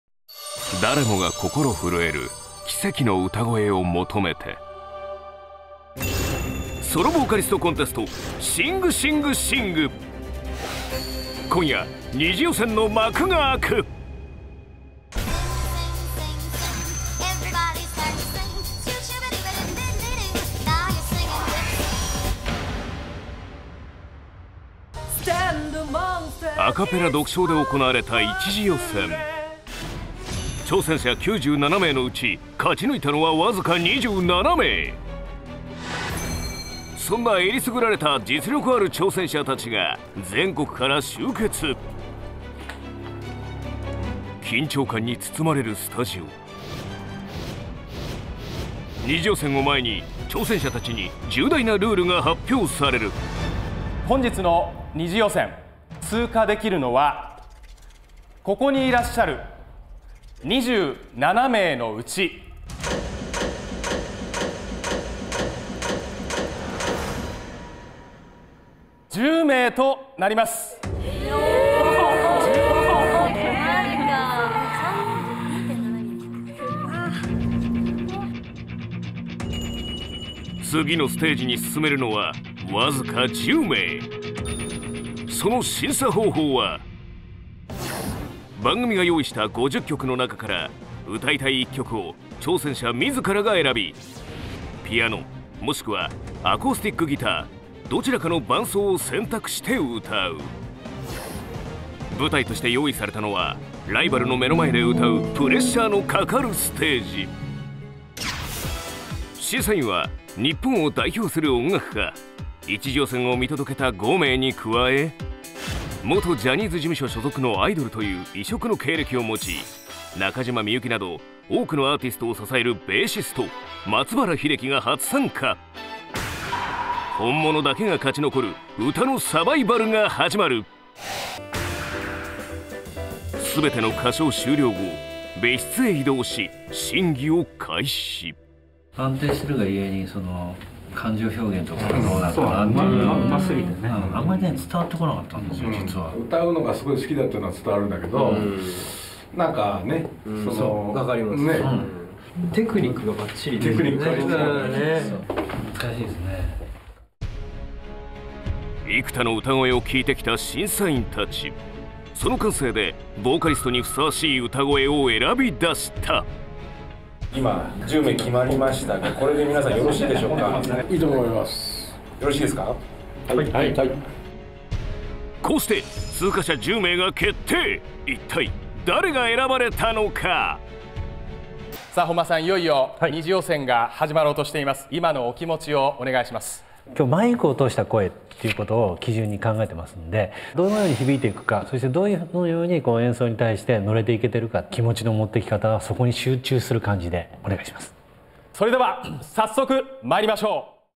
029_Sing!Sing!Sing!とは②
97名が挑んだ「一次予選」で27名に絞られた「Sing!Sing!Sing!」。
次なる各ステージ「二次予選」について、審査方法などルールを、ちょっとご紹介しましょう。